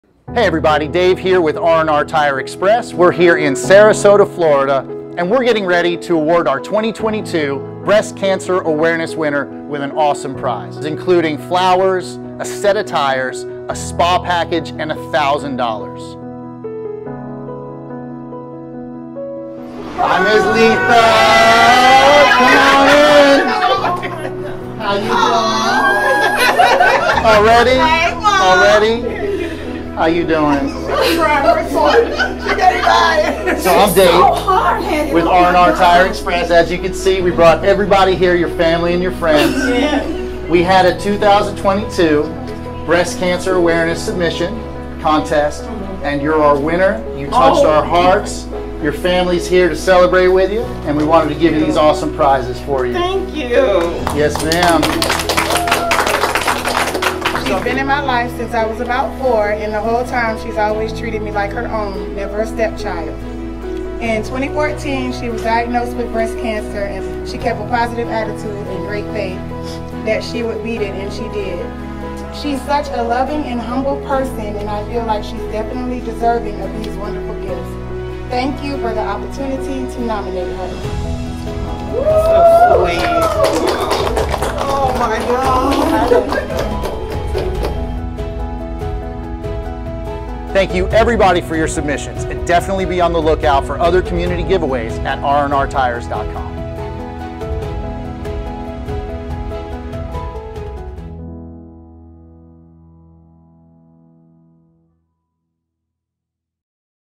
Breast Cancer Awareness Giveaway WINNER | RNR Tire Express
October is Breast Cancer Awareness month and RNR Tire Express had the wonderful opportunity to honor the sweetest survivor, Leatha. We received thousands of entries and were thrilled to celebrate her a surprised her with a new set of Tires, a VIP Spa Day Package and $1,000 in cash! Be on the lookout for more community giveaways at RNRtires.com. Think Pink!